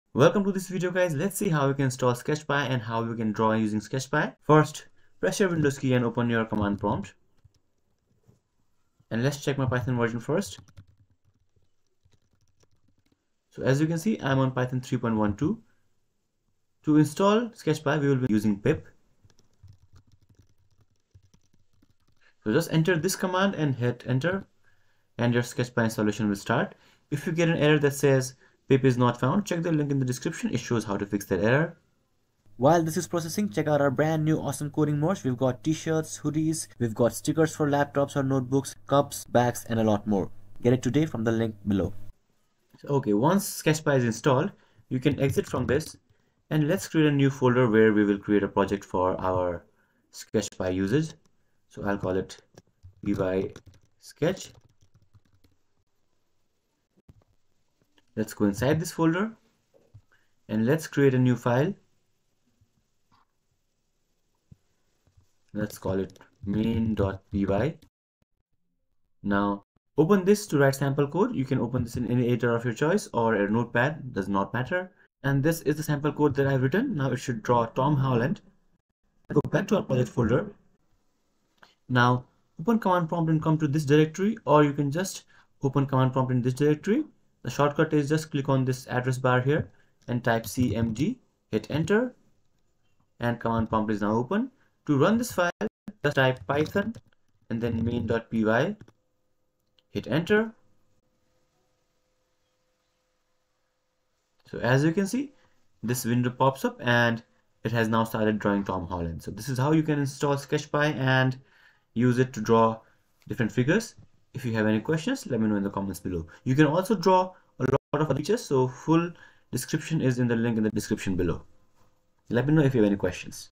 How to Install SketchPy in Python 3.12 - Draw in Python (2024)
In this video, I'll show you how you can install sketchpy in python 3.12, and how you can draw using python.
We will use PIP to install sketchpy. If you don't have pip installed, or you're getting a pip error. Check the video below to install PIP.

By the end of this video, you'll have installed SketchPy in python 3.12, and you'll have learnt how to draw using python using sketchpy.